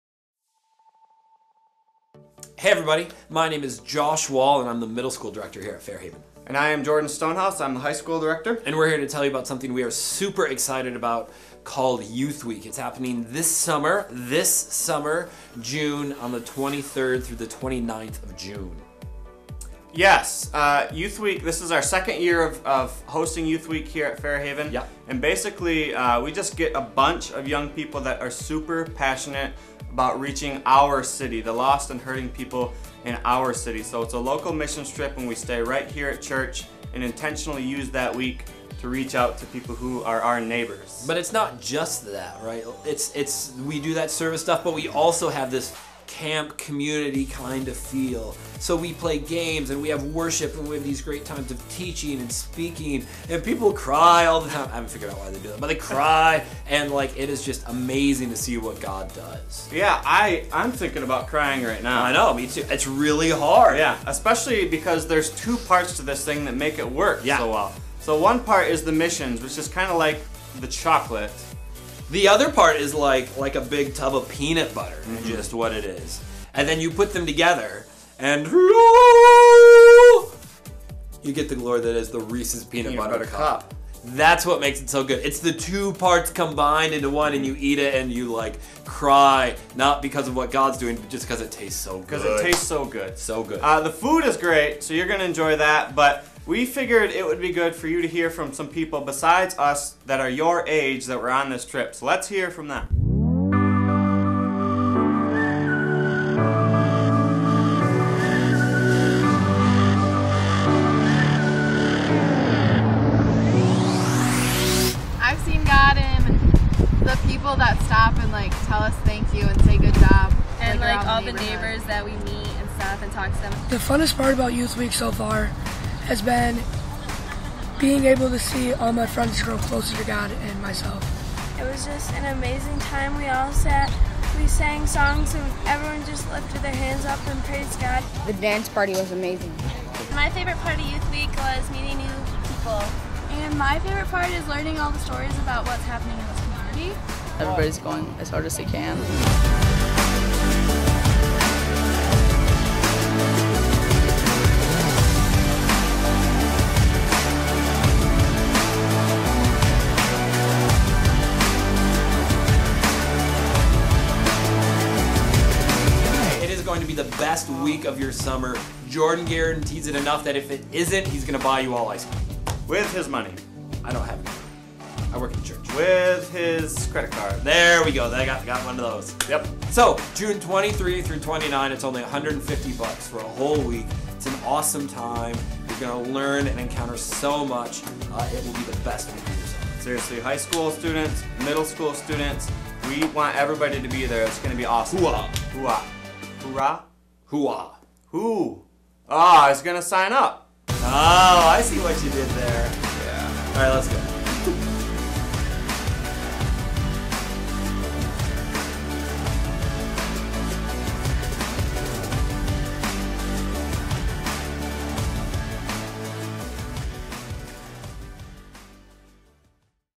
What is Youth Week?
If you're in high school or middle, you are miss this! Fair Haven Church is hosting Youth Week 2013 from June 23-29! Youth Week is a local missions trip, packed with so much more (like a a reeses peanut butter cup!).